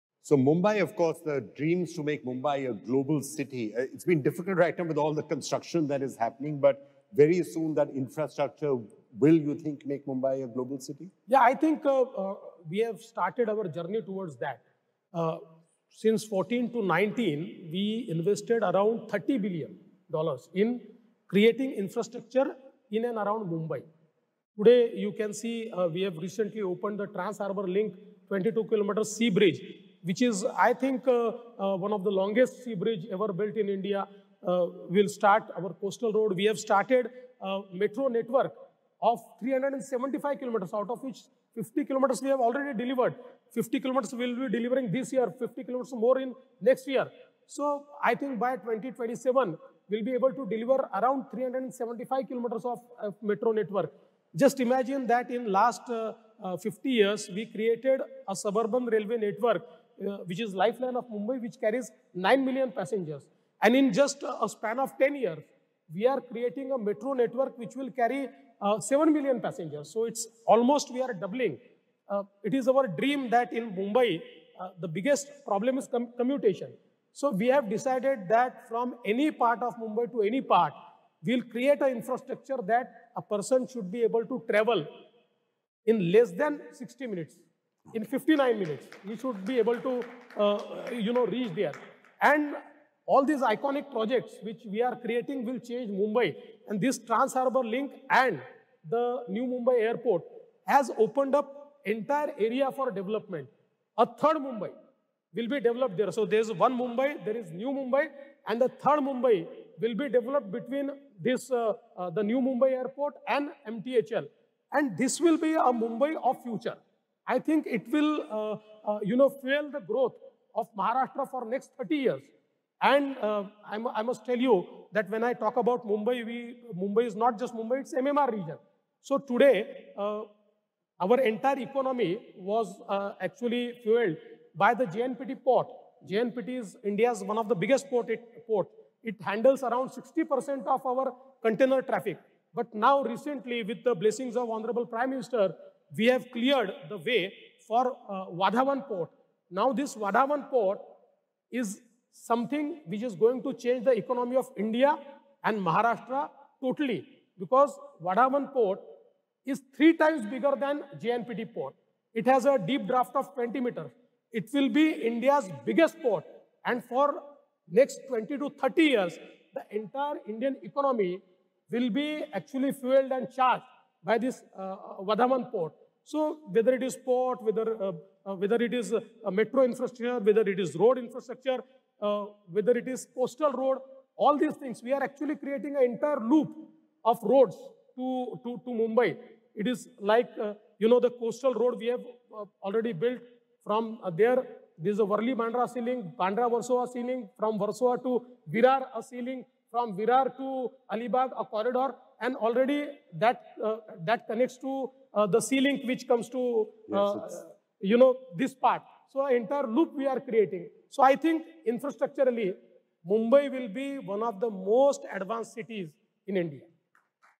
Vadhavan Port will change the economy of India and Maharashtra | Mumbai | DCM Devendra Fadnavis
(India Global Forum - Mumbai 2024 | Mumbai | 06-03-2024)